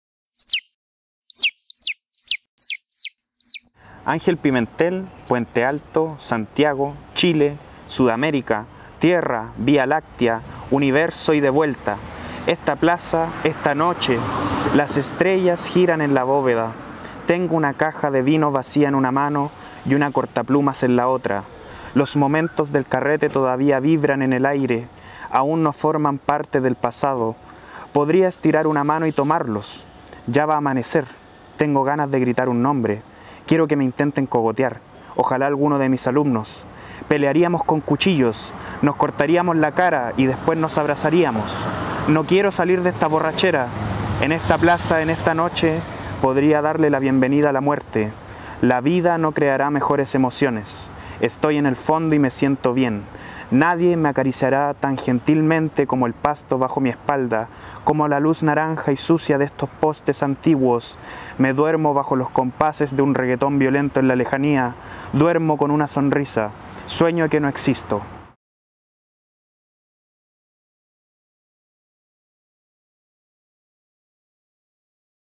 Lectura - PROFESOR SÍSIFO de Álex Saldías
💼 El relato de un profesor egresado que ve cómo las ilusiones de la docencia se convierten en burocracia dentro de un sistema educacional injusto, es el tema central de PROFESOR SÍSIFO, la segunda novela del escritor Álex Saldías.